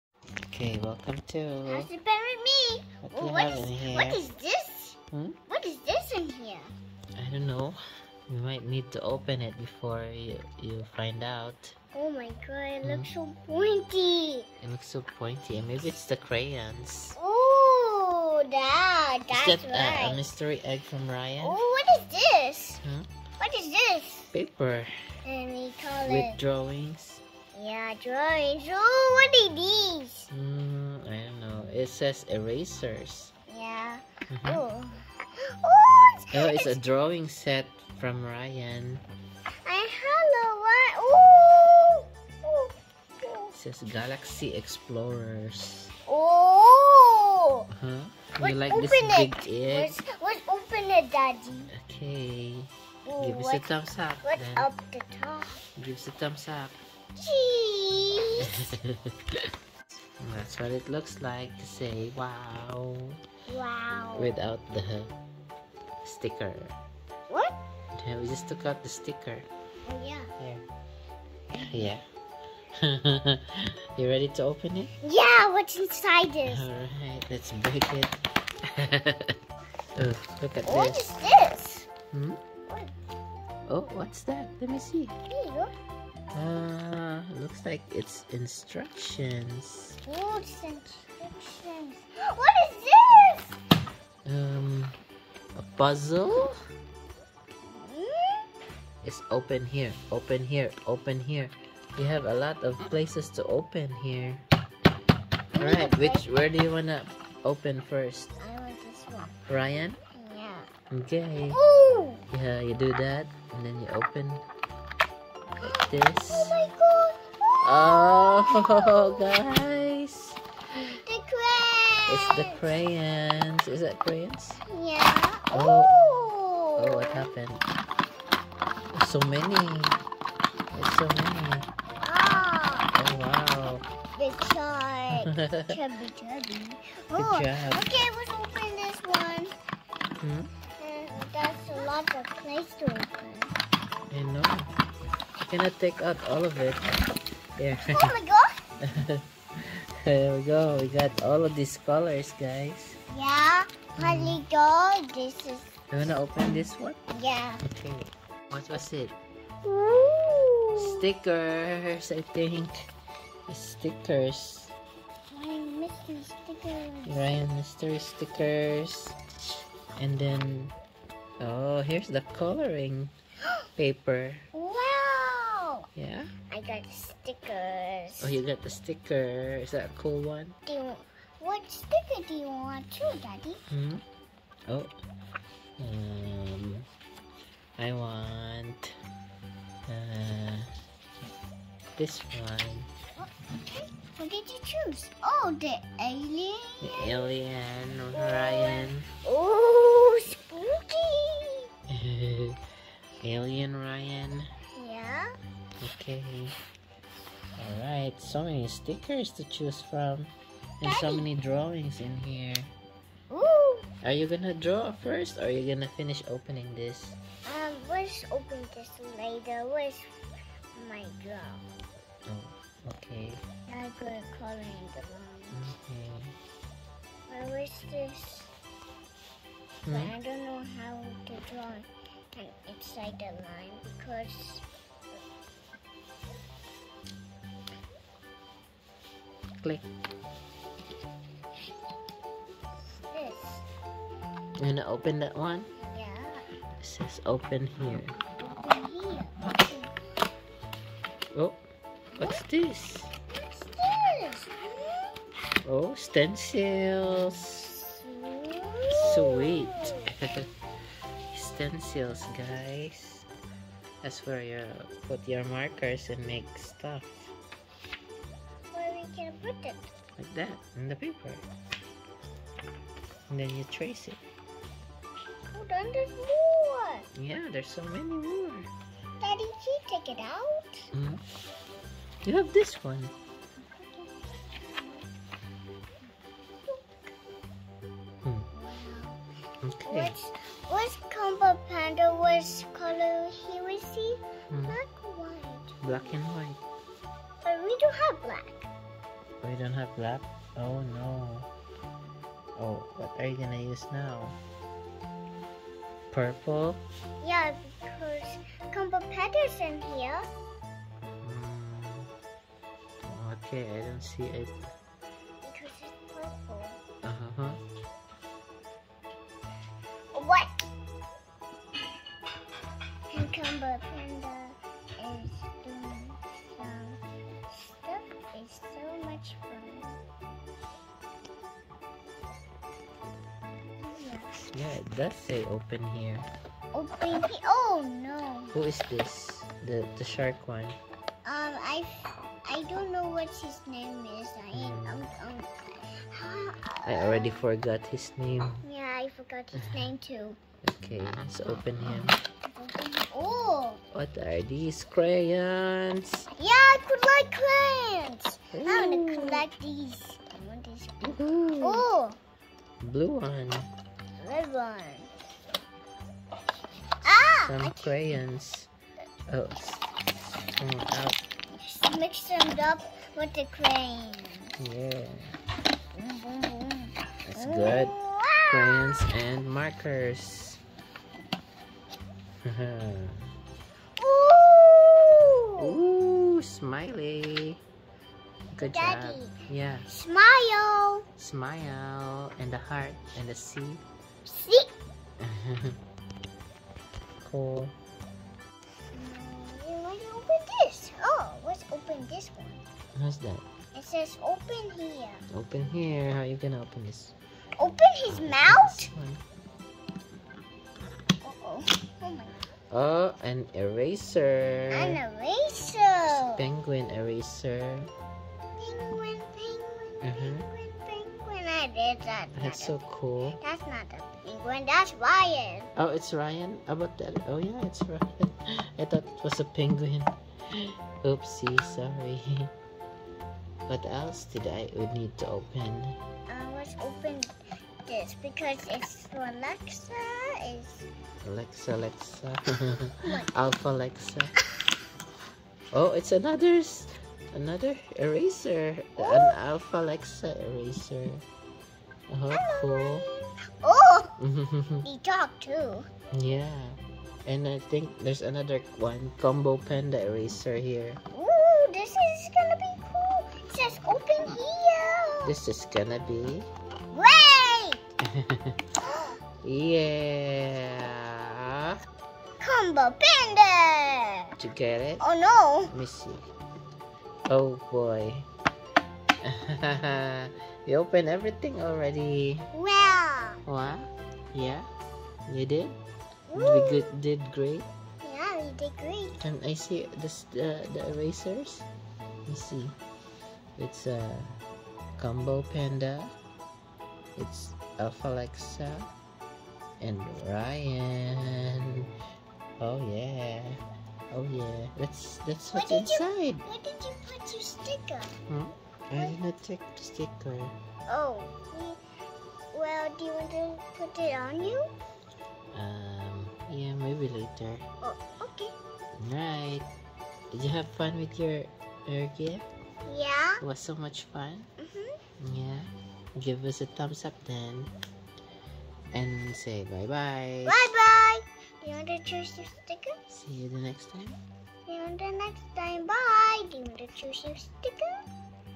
Opening Ryan's Mystery Egg For Art And Drawings
Opening Ryan's Mystery Egg For Art And Drawings. Welcome back to House of Penn with Zee!

Zee wanted this mystery egg as a gift for Christmas so we got her one.